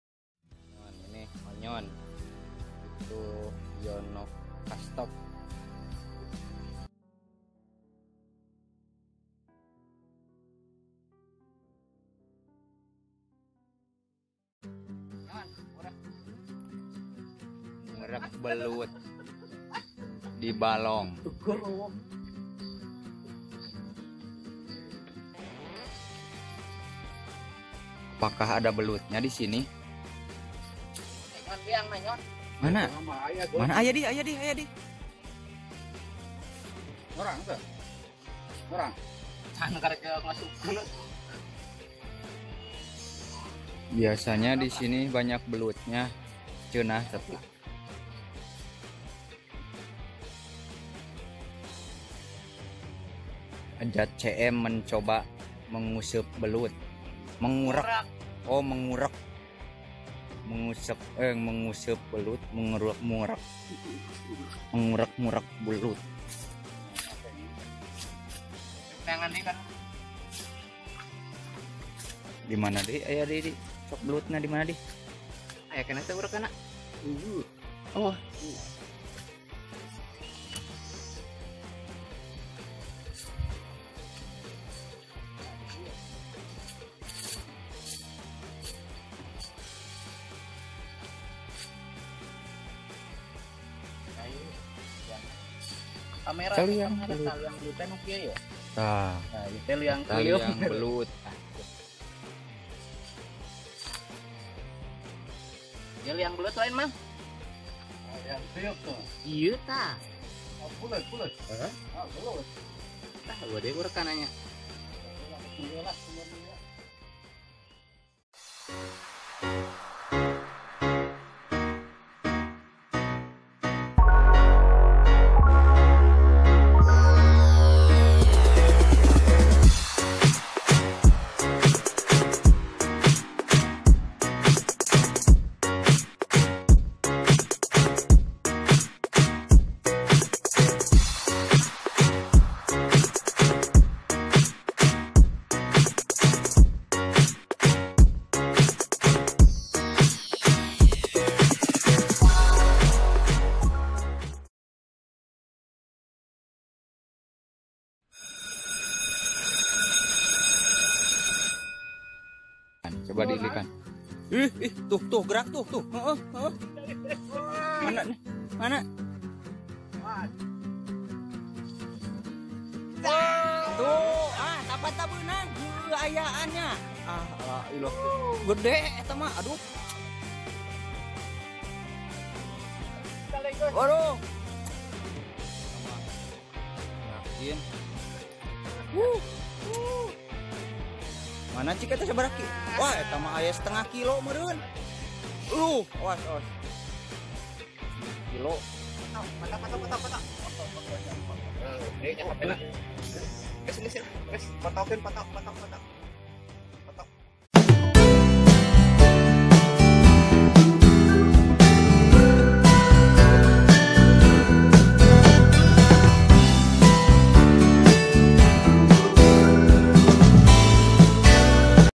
Mancing - Ngurek Belut Gede di Balong Si Replay
mancing belut atau ngurek belut bersama si replay di balong.
belutnya lumayan gede-gede,  rata-rata seberat setengah kg.